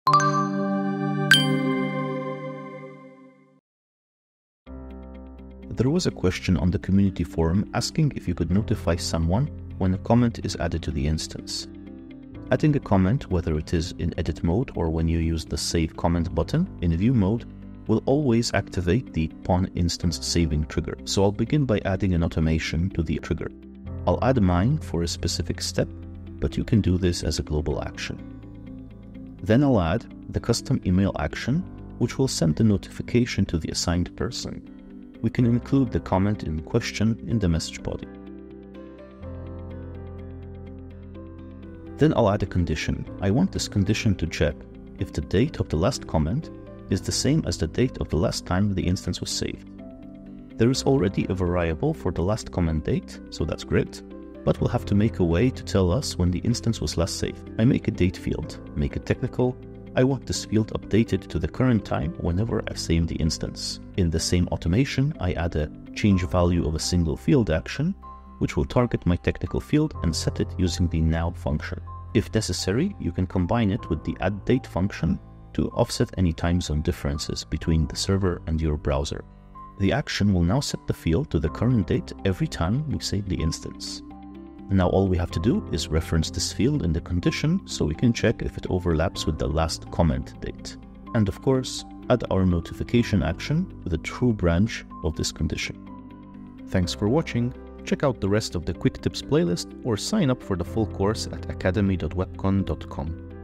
Comment Notification |💡Quick Tips
Want to know when a user makes a comment on the form? It is very easy to set up an action that is triggered by the instance being saved.